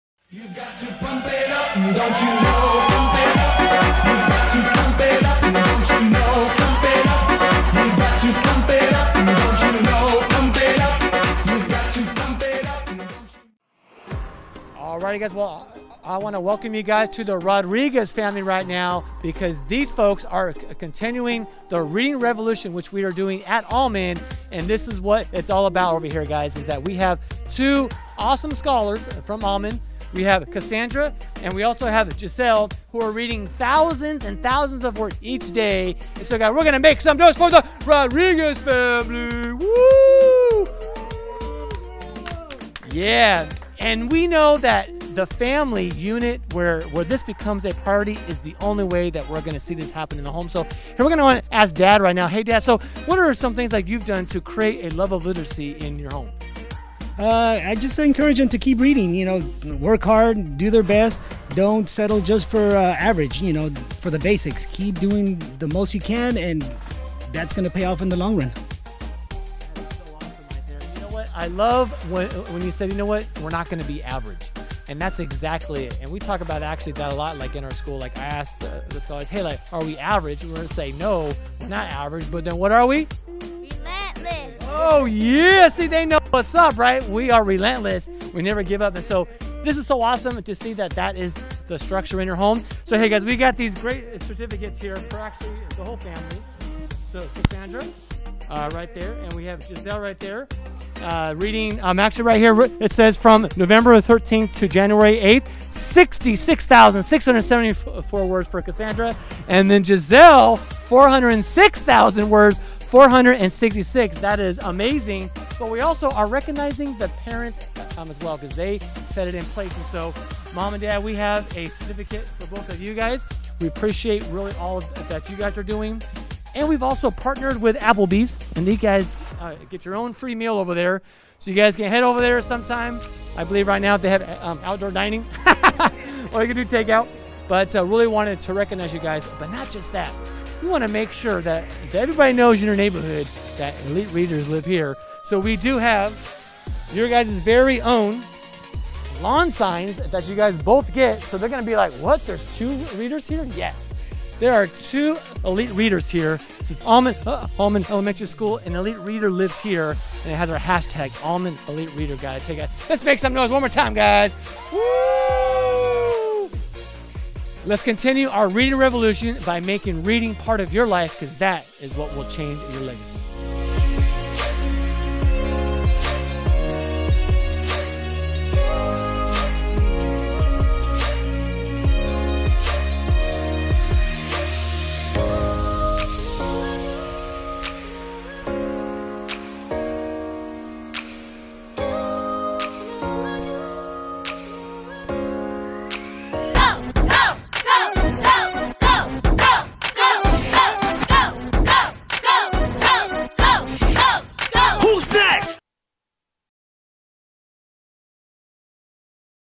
Almond Elite Reader Recognition - Mr. and Mrs. Rodriguez
Almond is super stoked to recognize Mr. and Mrs. Rodriguez for creating a love of literacy in their home.  Their home has not 1, but 2 Elite Readers!!! Teamwork has really helped this family in achieving such valuable goals.